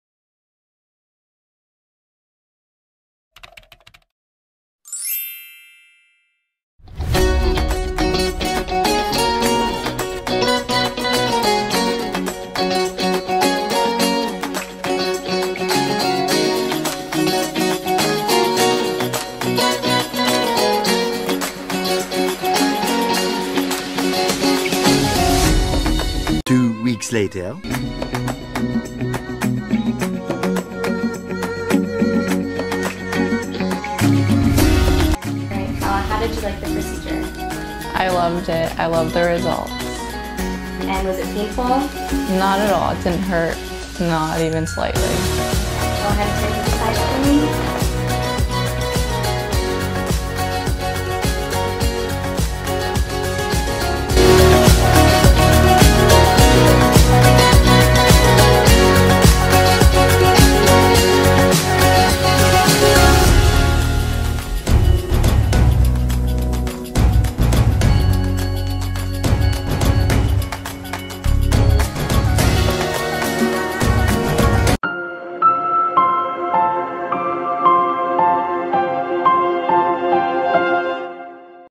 Non-Surgical Chin Augmentation Follow-up & Testimonial
This young patient desired  more balance to her facial features of the volume in her cheeks to balance out the lower portion of her face, she had concerns of facial asymmetry and a recessed chin. Dr. Mira spoke with the patient and mapped out what would get her to her desired goals. She used 1.5mL of Radiesse to achieve this result. She shares her results with us, and what she thought about the experience. All procedures performed by Dr Mira Mahajan at Golden Glow Medical Spa serving the Bellair Bluffs area in Florida.